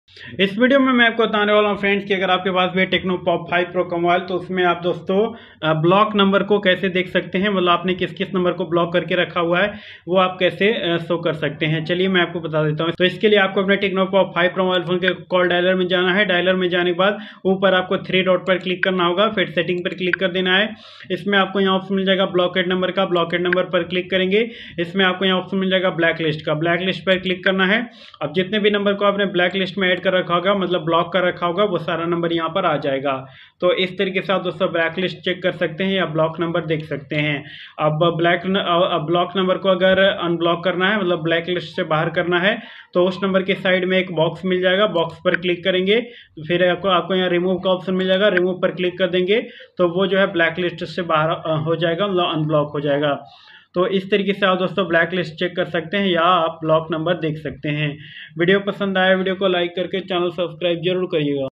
tecno Pop 5 pro me block number kaise dekhe yah kaha hota hai। tecno Pop 5 pro blacklist check kaise
is video me bataya gaya hai friends ki tecno pop 5 pro me block number kaise dekha jata hai matlab blacklist check kaise kiya jata hai